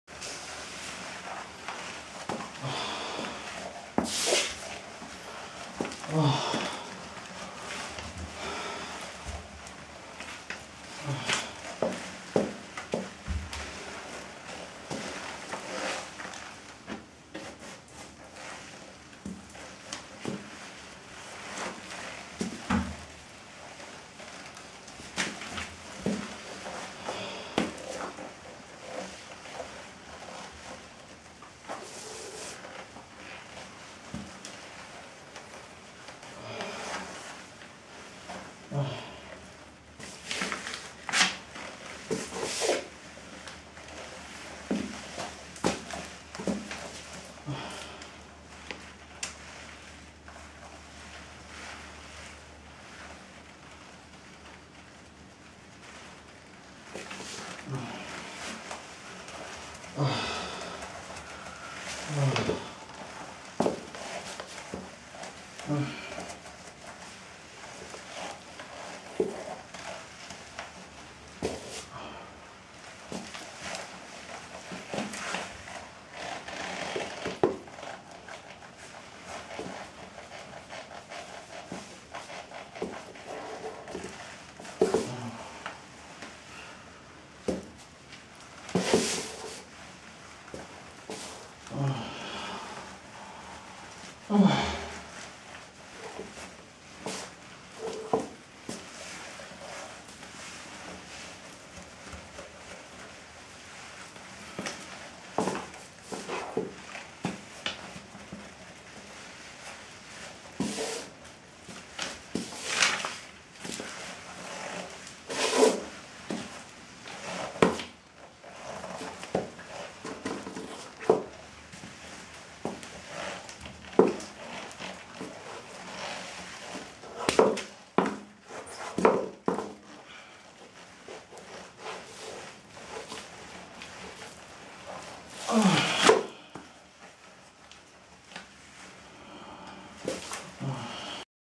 Muddy celebration of 2017 - Part 4
After the muddy exploration I decided to let another gorgeous pair of patent leather shoes try some taste of mud... Here you go!